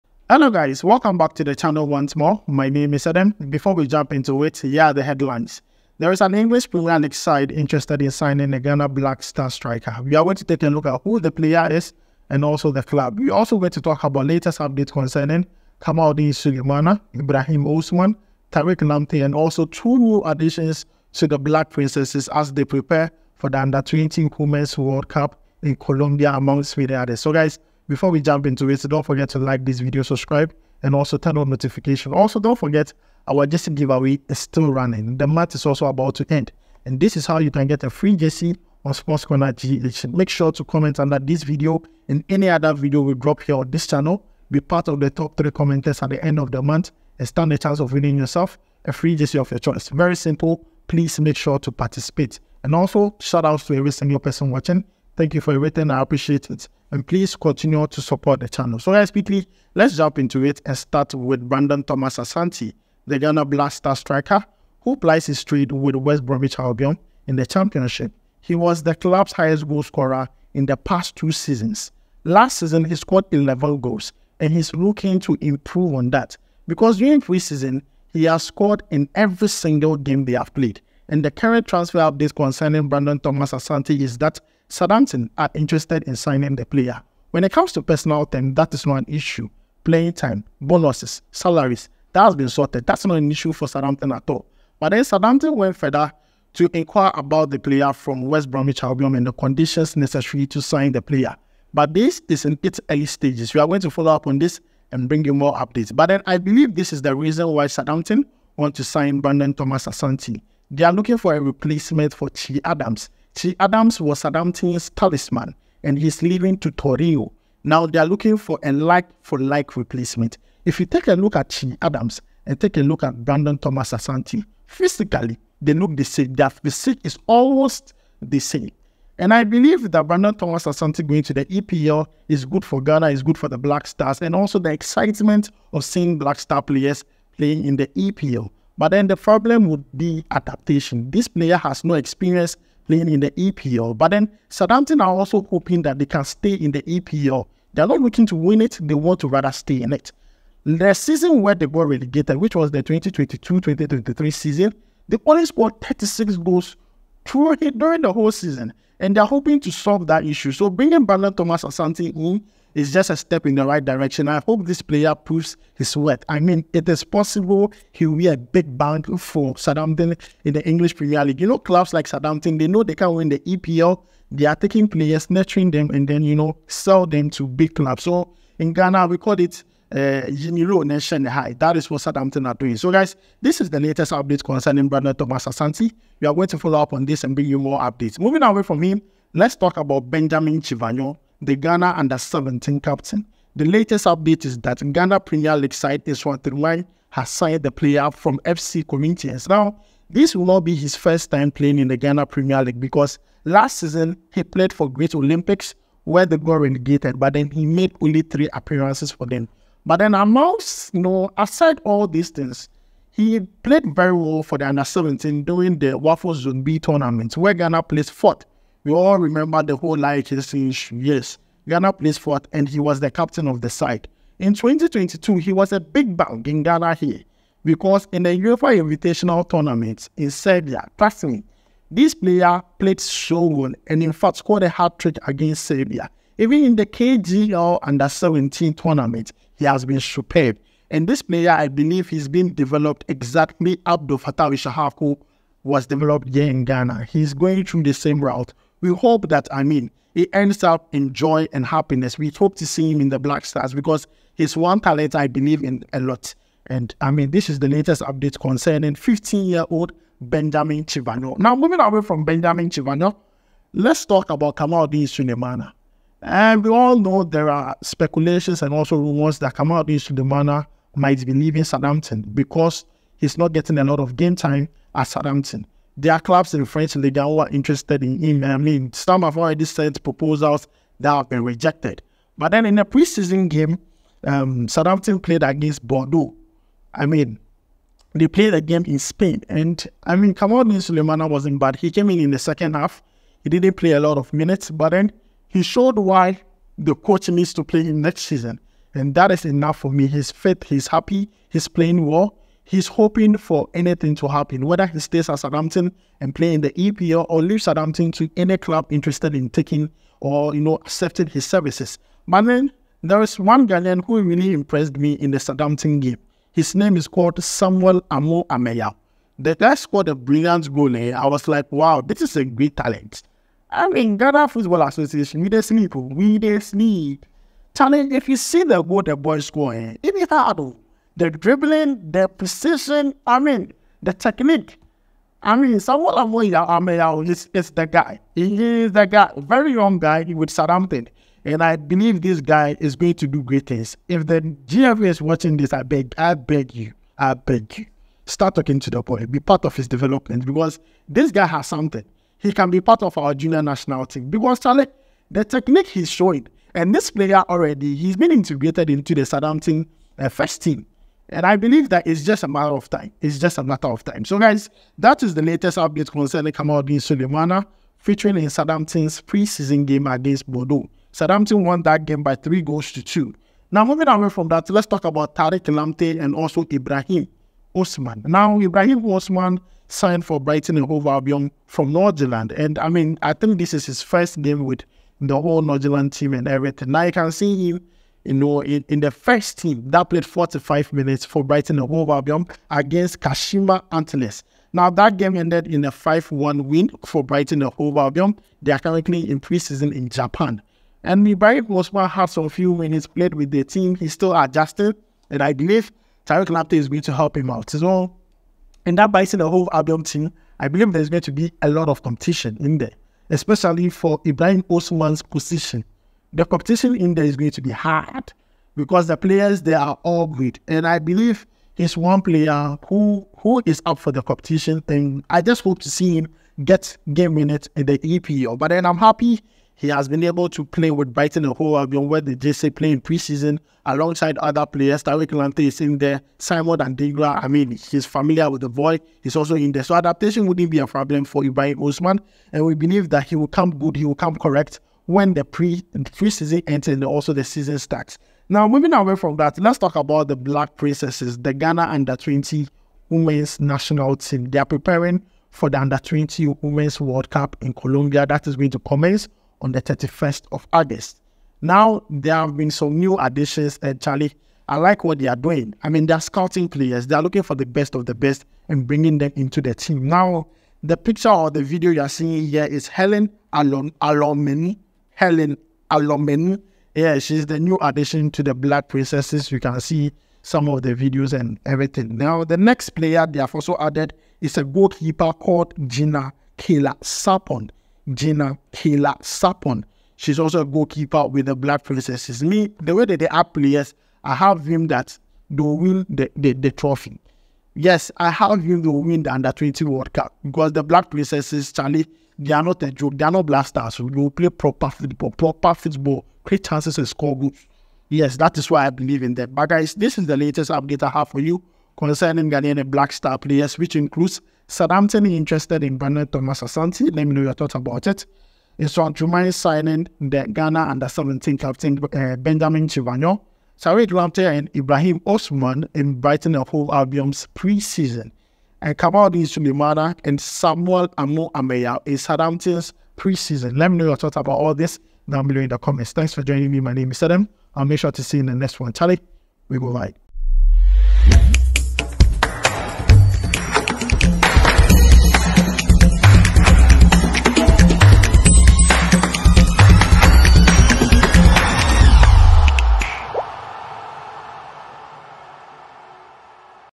GOOD NEWS🇬🇭 NEW BLACK STARS STRIKER TRANSFER…TARIQ LAMPTEY, KAMALDEEN & IBRAHIM OSMAN UPDATE
Ghana black stars news on Brandon Thomas-asante transfer to EPL side Southampton and Leicester plus Spanish Laliga side Espanyol, kamaldeen Sulemana pre season game for Southampton, tariq lamptey and Ibrahim Osman brighton Vs kashima antlers

SportsCorner gives you up to date information on football and other sports related stories(mainly ghana black stars and english premier league).